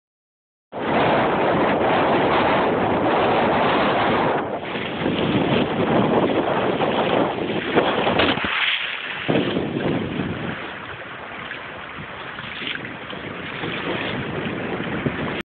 CAVOK. Jouet Golif.
Got a wet phone while filming sailing "CAVOK", my Golif22, in a breeze on Wolderwijd. Deze video is geüpload vanaf een Android-telefoon.